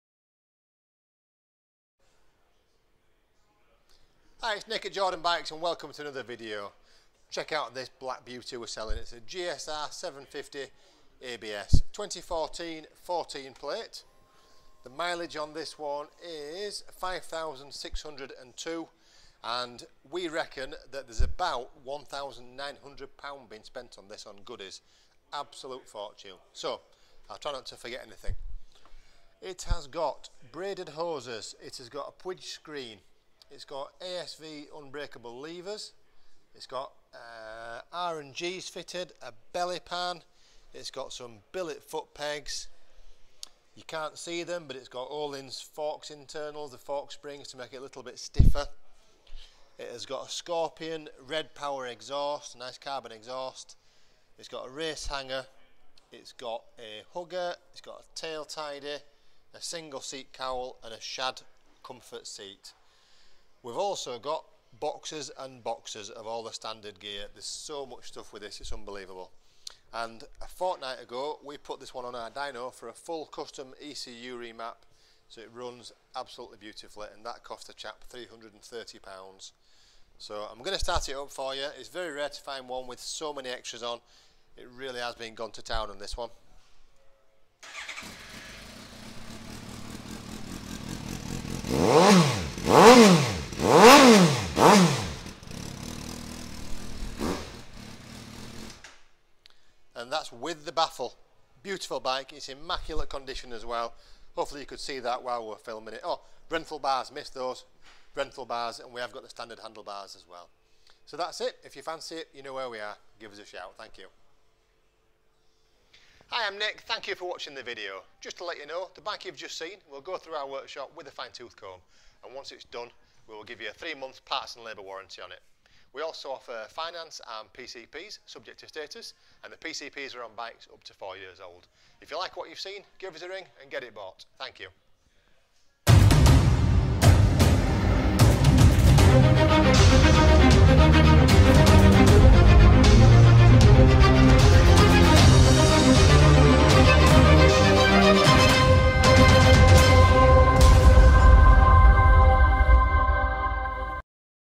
Jordanbikes- for sale Suzuki GSR750 ABS 2014/14 5602 miles Huge extras list - please read the full
FOR SALE - SUZUKI GSR750 2014/14 - This is an immaculate bike and it's only done 5,602 miles  with an absolute fortune spent on it, "Lets take a deep breath and go through the goodies". It has a Scorpion Red Power GP exhaust, race hanger, tail tidy, Shad comfort seat, hugger, billet pegs, ASV levers, Renthal bars, Puig screen, seat cowl, bellypan, R&Gs, Ohlins fork internals, braided hoses, and we have remapped the ECU just a couple of weeks ago so that it runs like a dream. The bike also has Datatag, we reckon there are £1900 of extras in total, and the bike comes with boxes of standard parts too.  The bike has just had new tyres only 100 miles ago, it's ready to go for only £5,290.